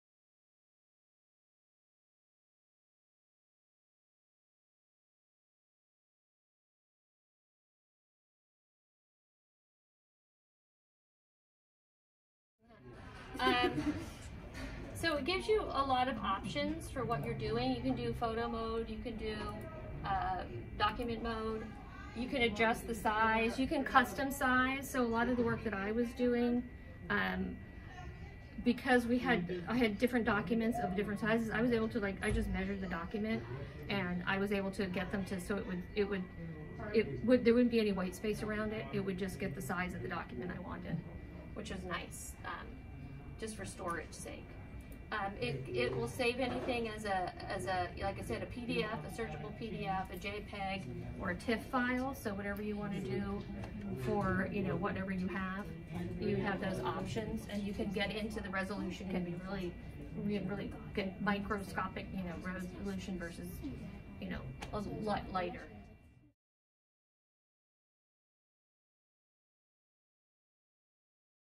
Watch now: Napa library opens "Memory Lab" DIY station for digitizing slides, photos, negatives and
Got old VHS, photos, slides or negatives? You can digitize them for free at the new Memory Lab at the Napa City County library on Coombs St. Take a look here.